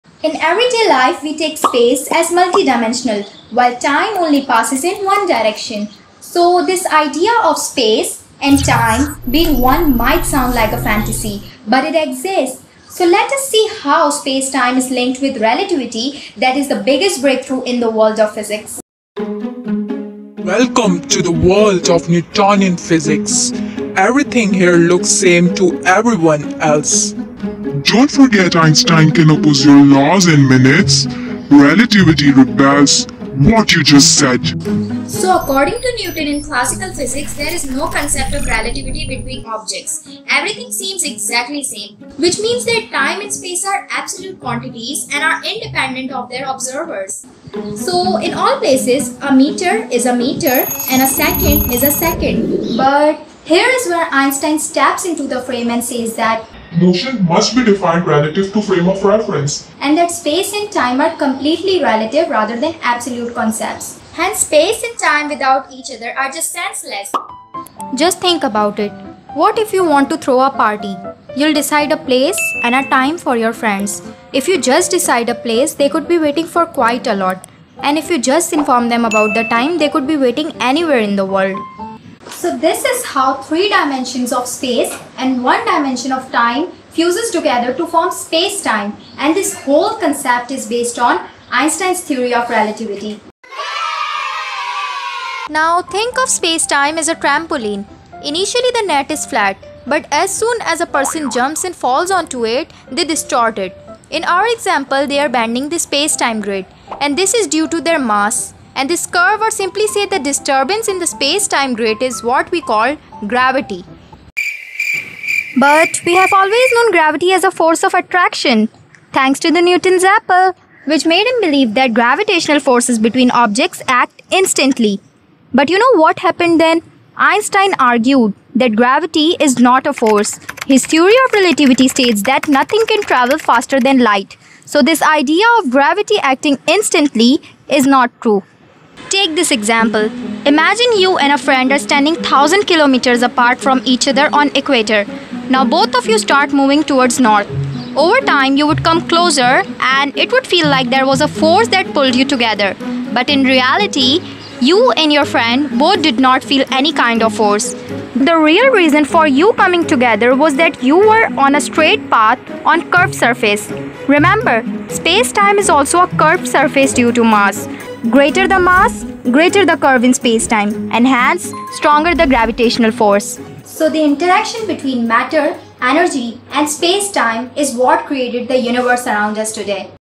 Space-time and Gravity: An application of Relativity || Breakthrough junior challenge 2021
HEY EVERYONE!
This is my contest entry for the breakthrough junior challenge 2021. It covers the topic of "space-time and how it results in gravity". Video aims to compare different concepts from classical and modern physics and conclude how ideas change over time. Space and time is a fundamental force of the universe.

Video Directed and scripted by me.